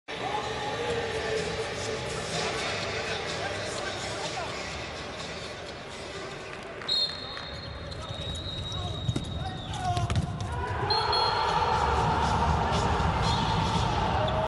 Action profile: Andy Schmid — Changing the rhythm and breaking through | IHF Education Centre
Changing the rhythm and breaking through from a standing position, by Andy Schmid